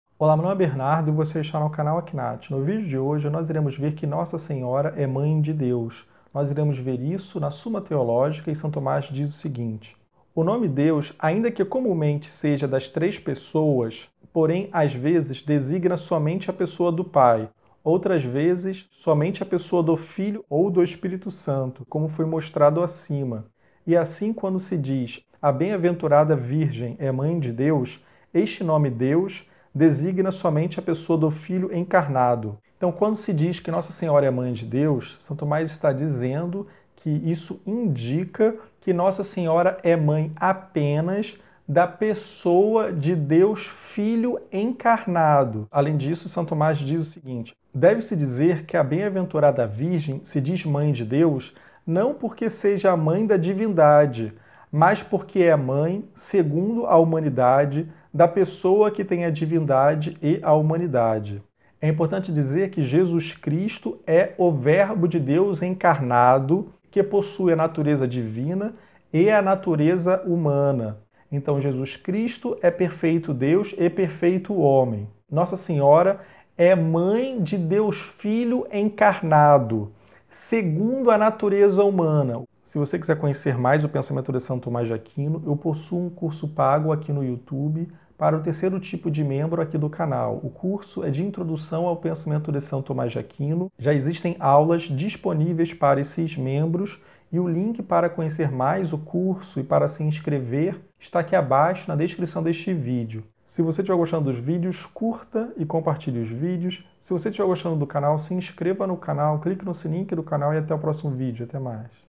DE QUE MODO NOSSA SENHORA É MÃE DE DEUS? - A visão de São Tomás de Aquino
Busca-se expor de que modo Nossa Senhora é mãe de Deus.

“O nome ‘Deus’, ainda que comumente seja das três Pessoas, porém, às vezes, designa somente a pessoa do Pai, outras vezes, somente a pessoa do Filho ou do Espírito Santo, como foi mostrado acima. E assim, quando se diz: “A Bem-aventurada Virgem é Mãe de Deus”, este nome ‘Deus’ designa somente a Pessoa do Filho encarnado.” (STh., III, q.35, a.4, sol.3)
“Deve-se dizer que a Bem-aventurada Virgem se diz Mãe de Deus, não porque seja mãe da divindade, mas porque é mãe, segundo a humanidade, da Pessoa que tem a divindade e a humanidade”. (STh., III, q.35, a.4, sol.2)

Livro tomado como base: TOMÁS DE AQUINO. Suma Teológica – volume 8. São Paulo: Loyola. 2ªed. 2009.

Suma Teológica – volume 8, Loyola – volume 4, Ecclesiae:
Link de compra da Suma Teológica (Amazon-Brasil) referente ao texto citado logo acima:
Editora Loyola
Editora Ecclesiae